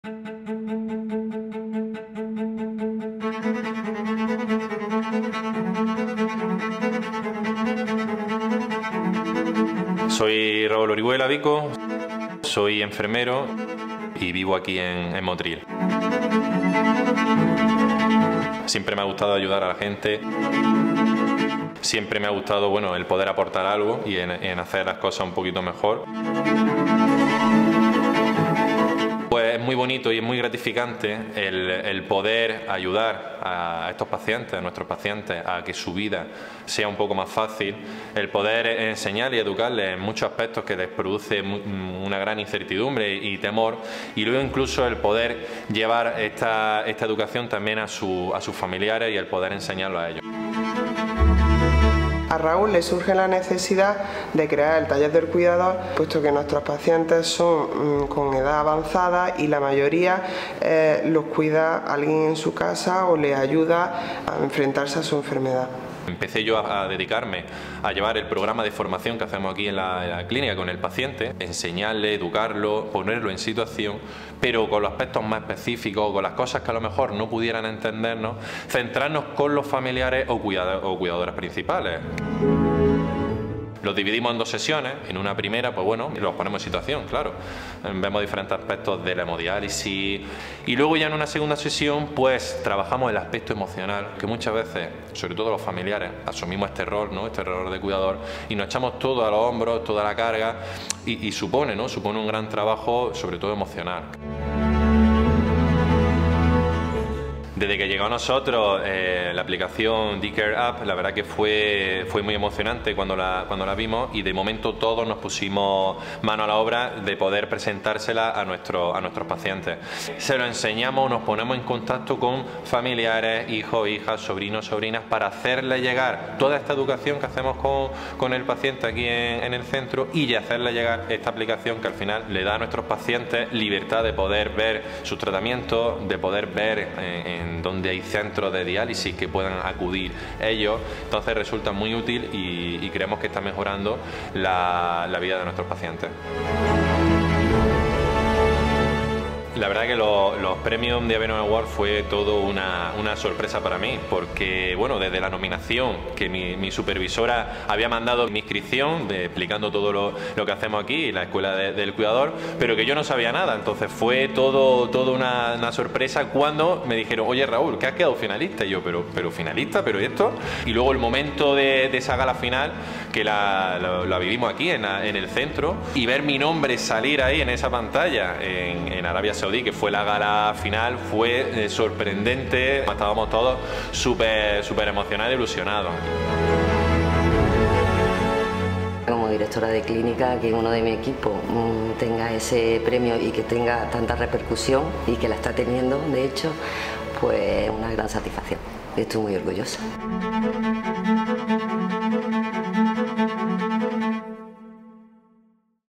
Meet Raúl Orihuela 2023 Diaverum Awards Winner in Inspiration category
Our 2023 Inspiration Award winner, Raúl Orihuela, Nurse at Diaverum Motril, Diaverum Spain shares his remarkable story.
Join us in celebrating passion, dedication, and the incredible impact Raúl has had on our DiaverumFamily and community.